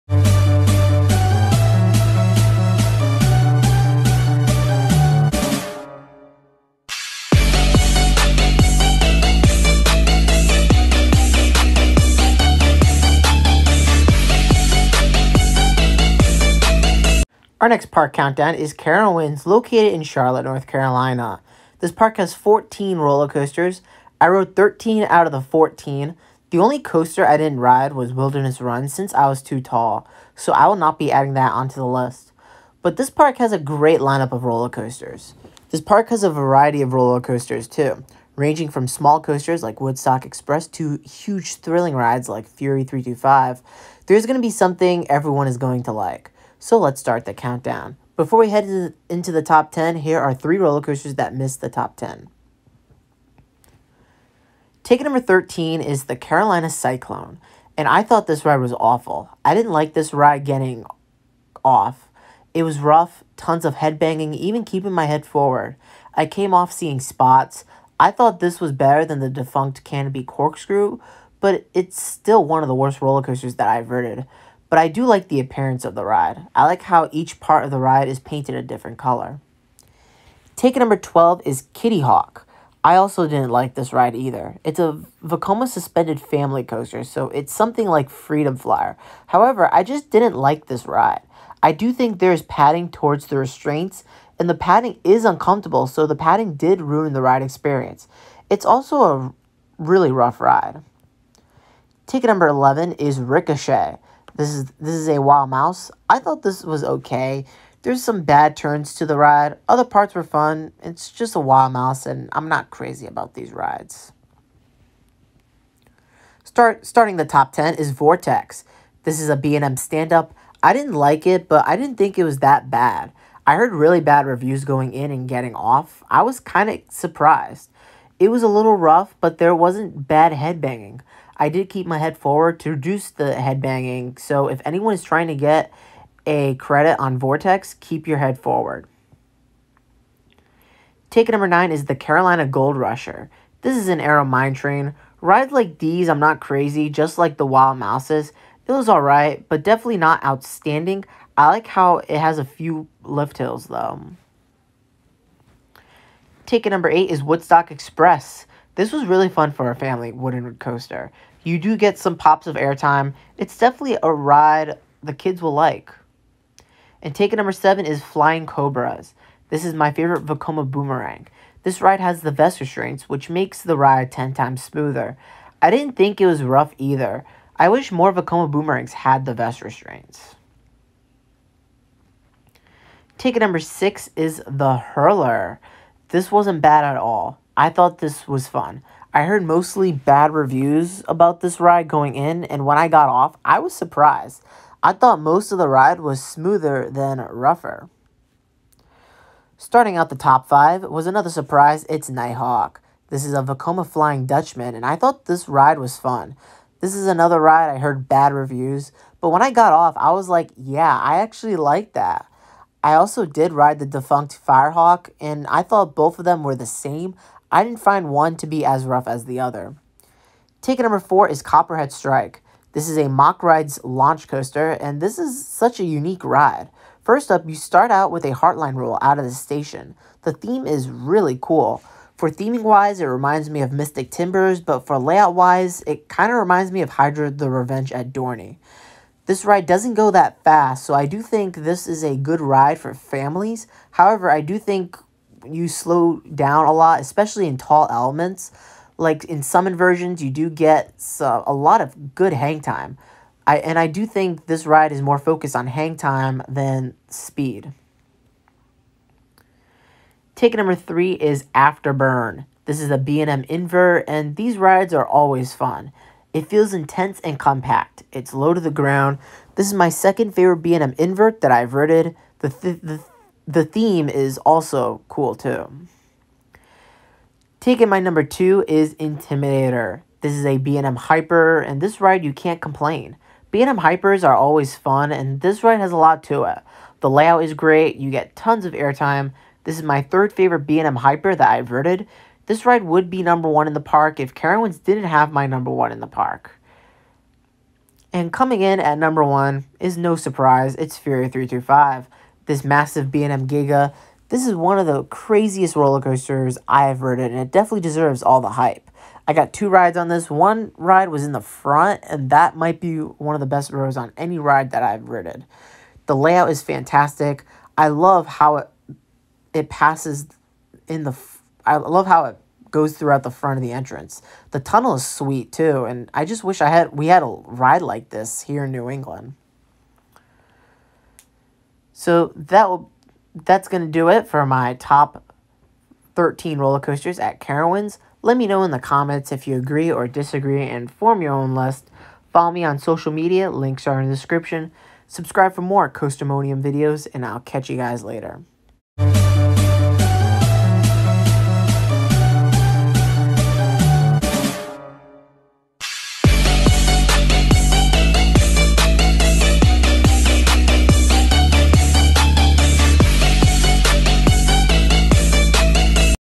Top 13 Roller Coasters at Carowinds
Here’s my Top 13 Roller Coasters at Carowinds! Let me know if you agree or disagree!